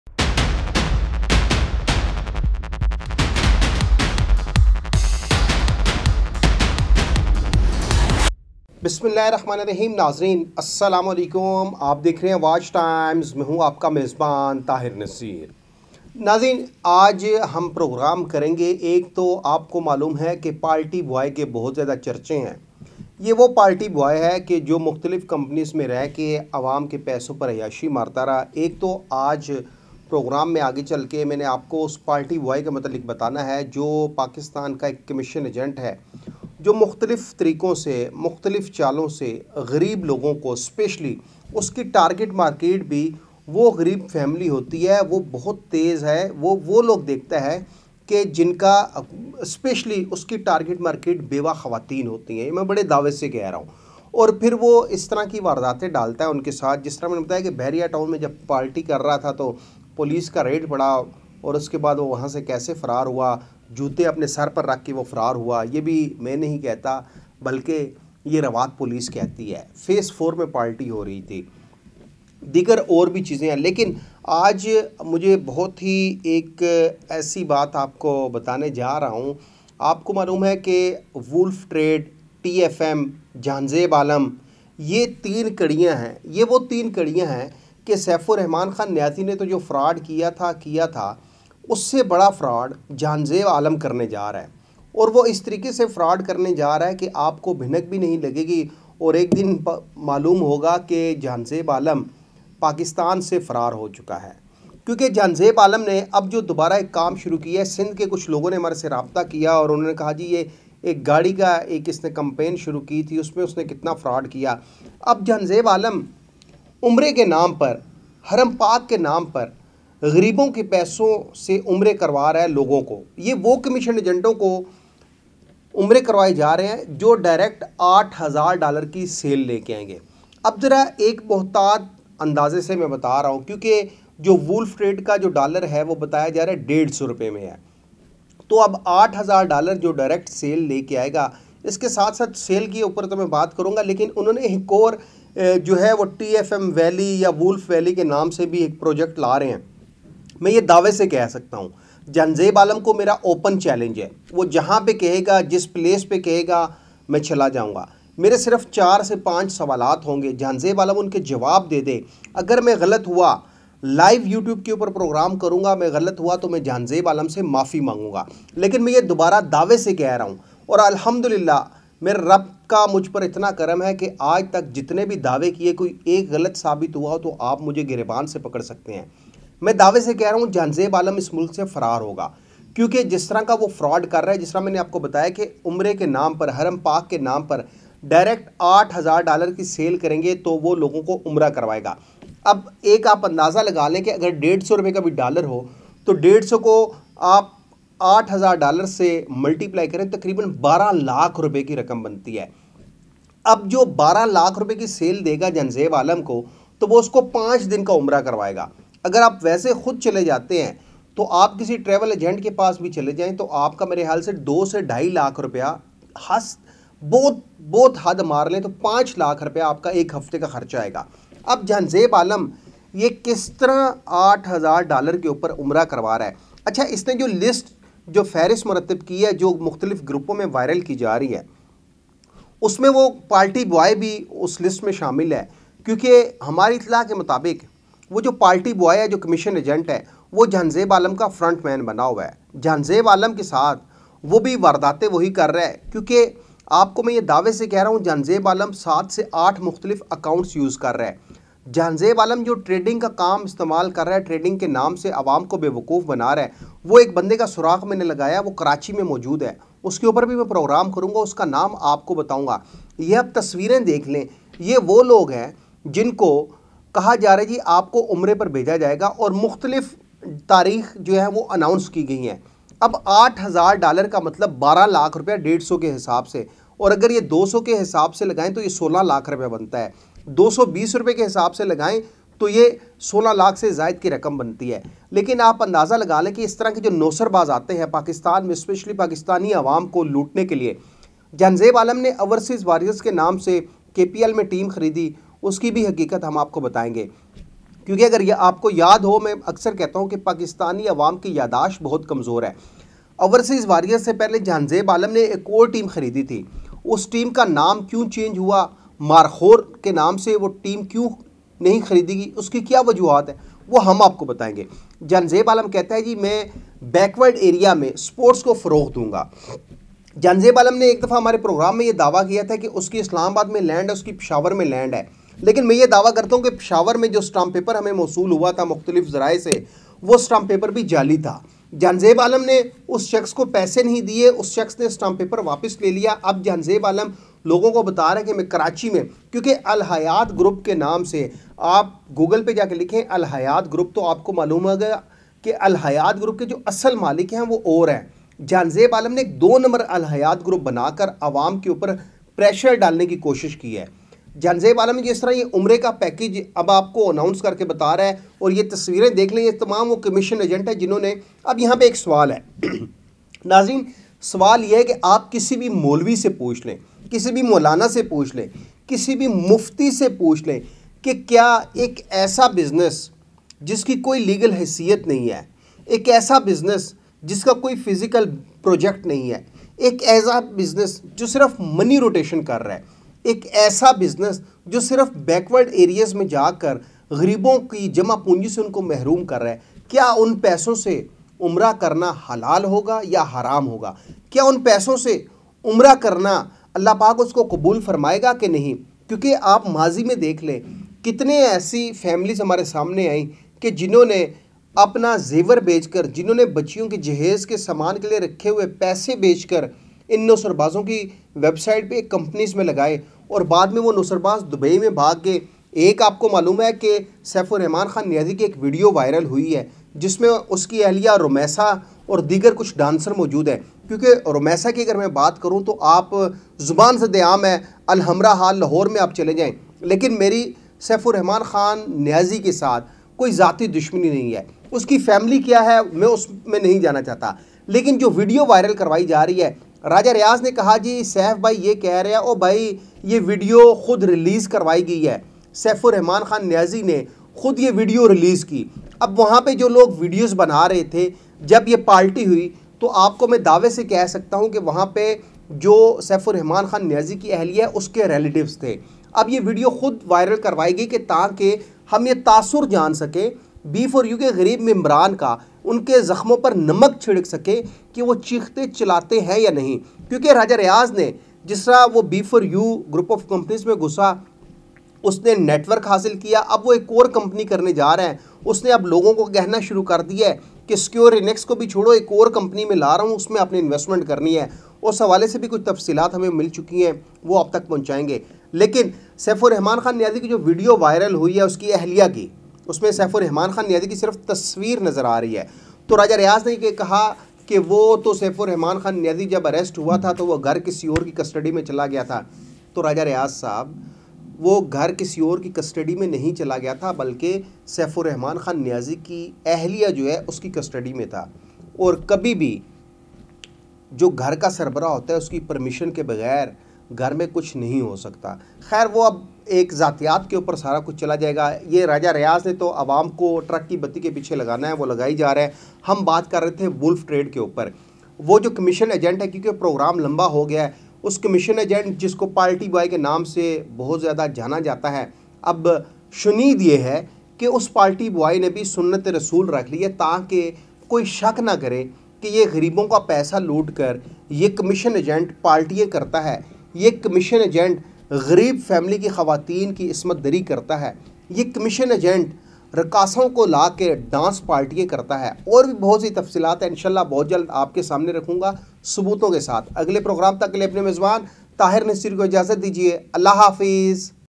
Wolf Trade Group Start Fraud | TFM CEO Jhanzaib Alam Review | Umrah Package By TFM Real Story
Wolf Trade Group Start Fraud TFM CEO Jhanzaib Alam Review Umrah Package By TFM Real Story Jhanzaib Alam Looting the innocents Pakistani TFM and WOLF Trade a Biggest Fraud in Network marketing Industry Wolf Trade TFM Not a Single Physical Project in all over the world Jhanzaib Alam plan to Collect huge amount for Pakistani people then left the Pakistan
Facebook Watchtimes